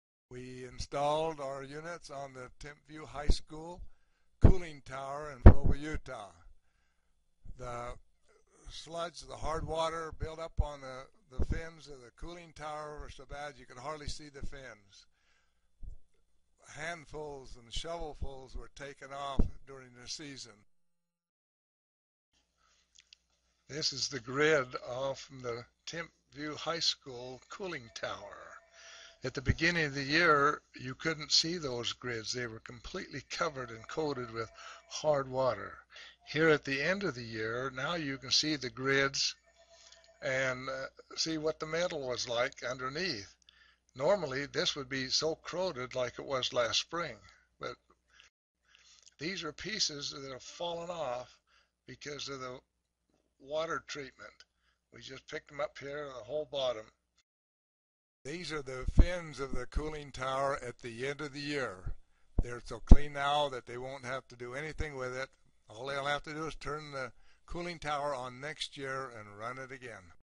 Commercial Water Softener Case Study
Commercial water softener case study at high school using Aqua Magnets for commercial water softening applications instead of a traditional salt-based water softener.